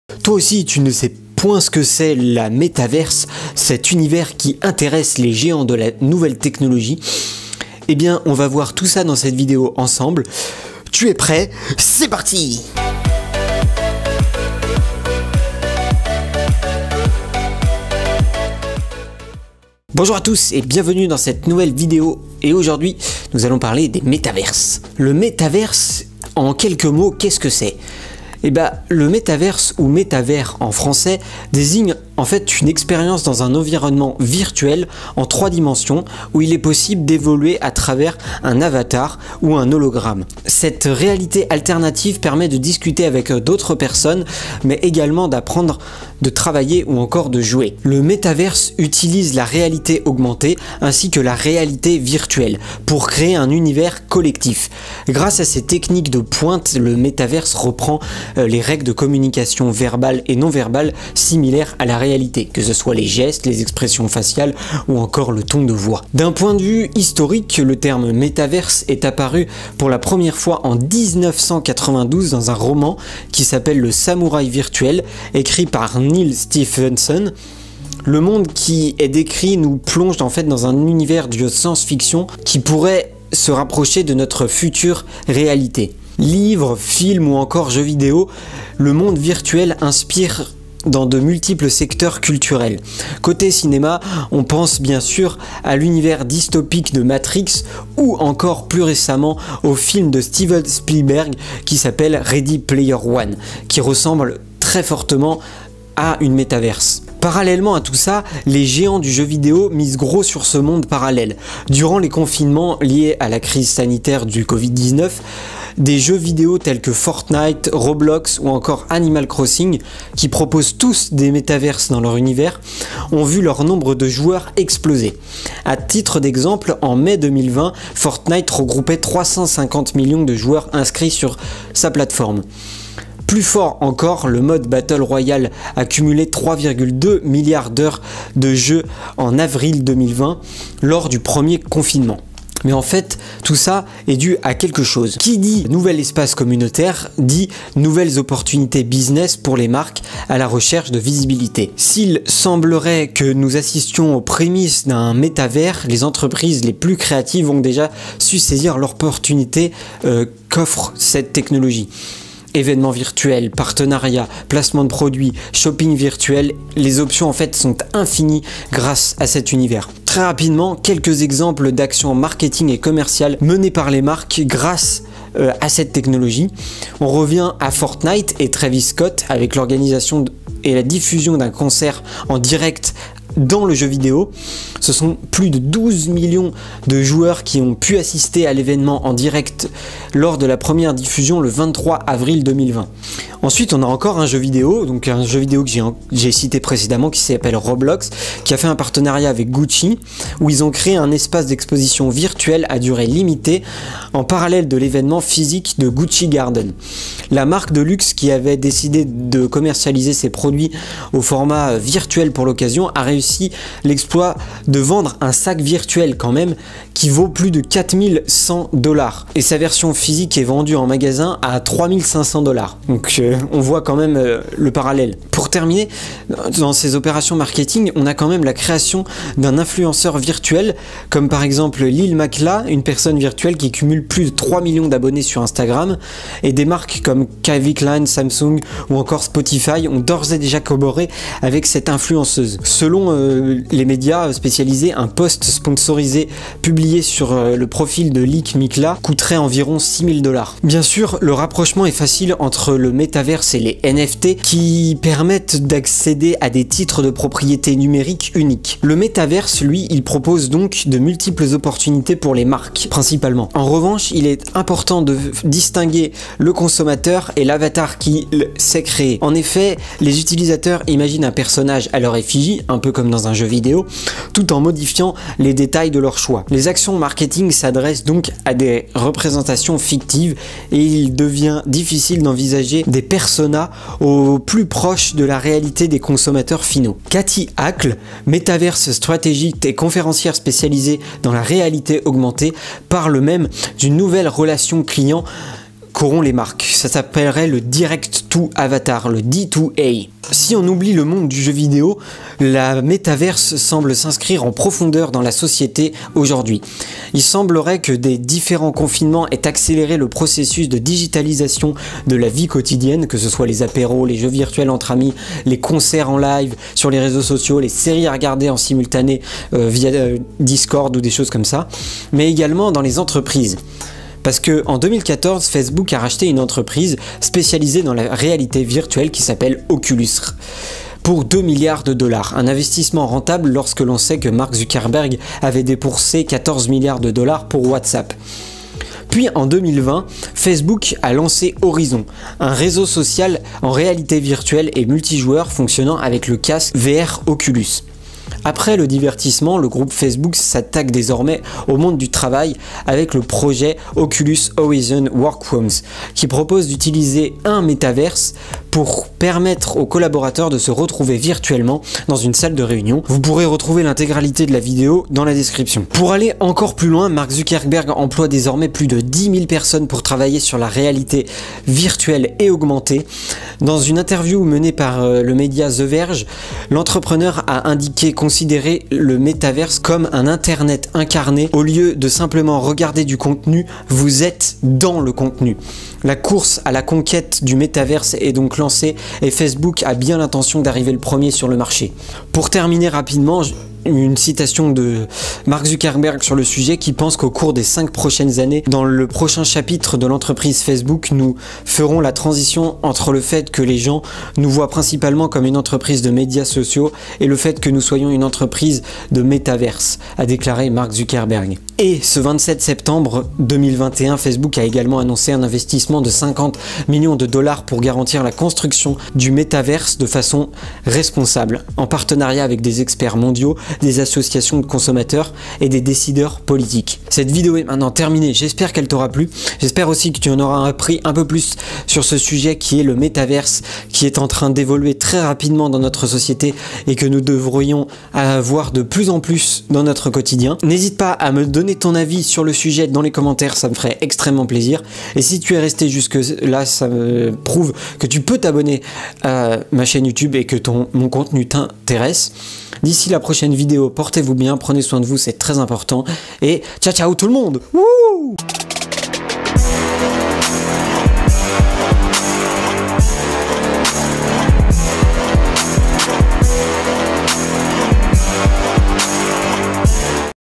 Metaverse : Cet univers qui affole les GAFA !
Le metaverse, cet univers parallèle qui intéresse les géants de la tech et plus particulièrement Facebook ! Décryptage de ce qui pourrait bien représenter l’Internet de demain.

⚠️ INFORMATION ⚠️

🌎 RESTONS CONNECTÉS 🌎

N'hésite pas à commenter cette vidéo pour donner ton avis, je lis tous les commentaires  ! 👀

📱 CHAINES HIGH-TECH SYMPA 🕹

MrApple 🍎